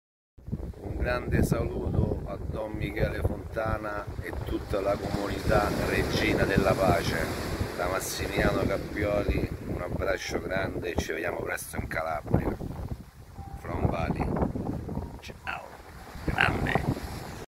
Saluto da Massimiliano Cappioli
Massimiliano Cappioli ha giocato in Roma, Cagliari, Udinese, Atalanta, Bologna, Perugia, Palermo, e in Nazionale maggiore.